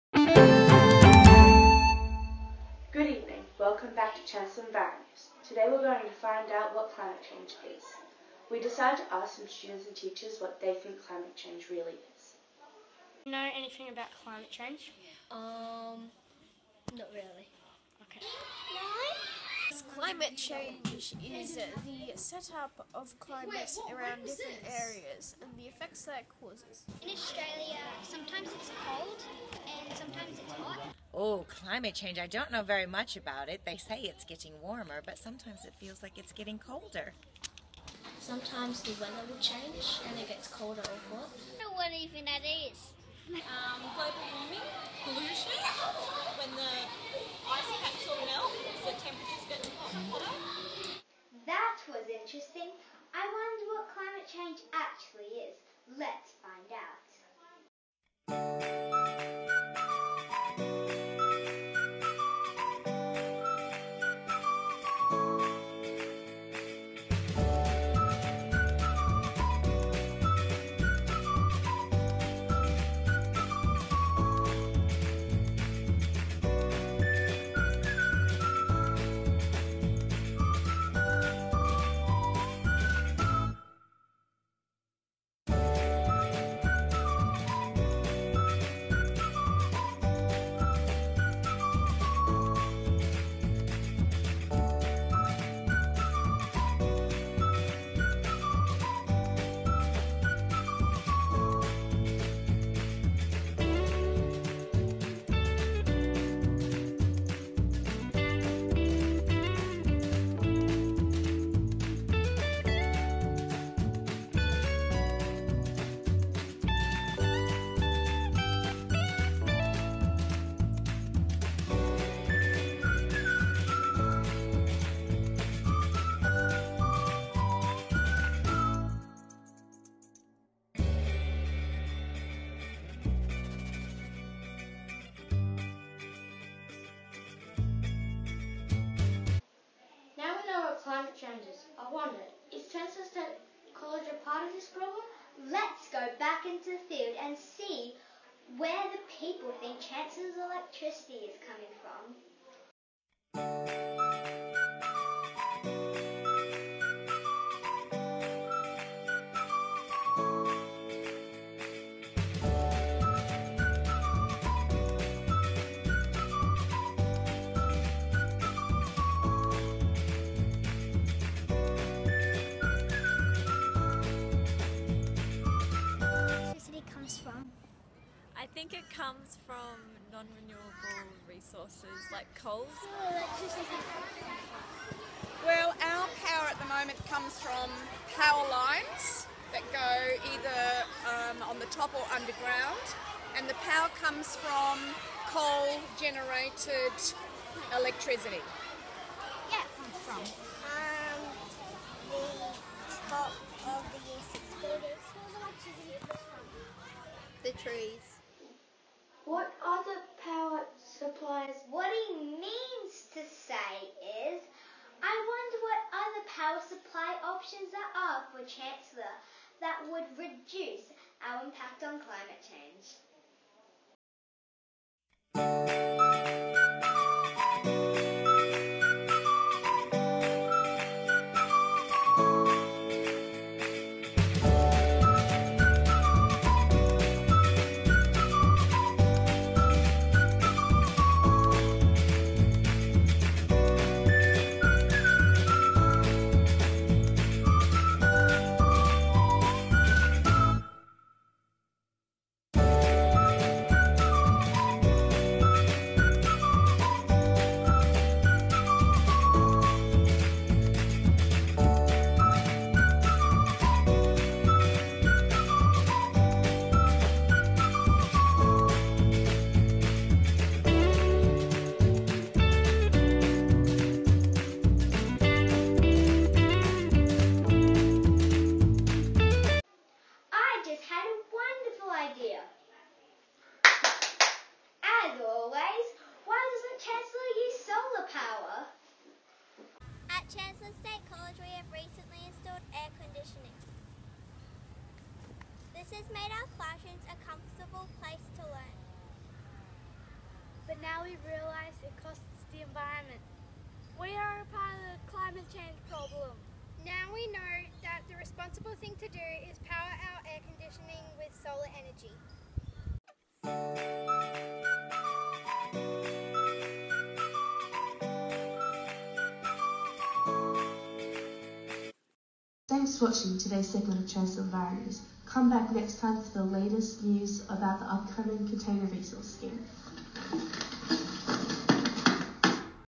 Kids in Action - Chancellor State College
The Environmental Commitee at the primary campus will be presenting their mini documentary at the Sunshine Coast Kids in Action Conference Friday September 7th. Students from more than 30 schools teaching each other about our impact on Climate Change.